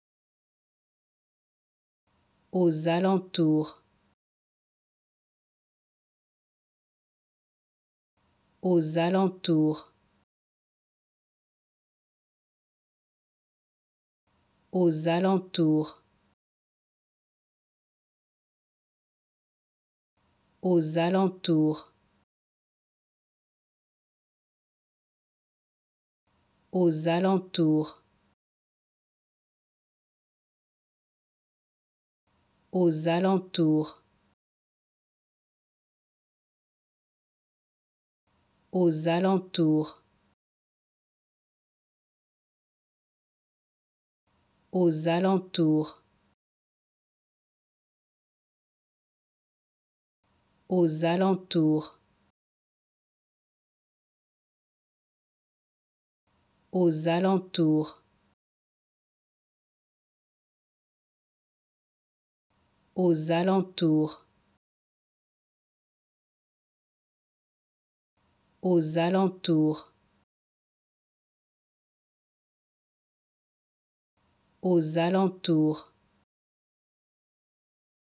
How to Pronounce ''Aux alentours'' (In the area) Correctly in French
Learn how to say and properly pronounce ''Aux alentours'' in French with this free pronunciation tutorial. Apprenez à prononcer des mots en français grâce à nos tutoriels.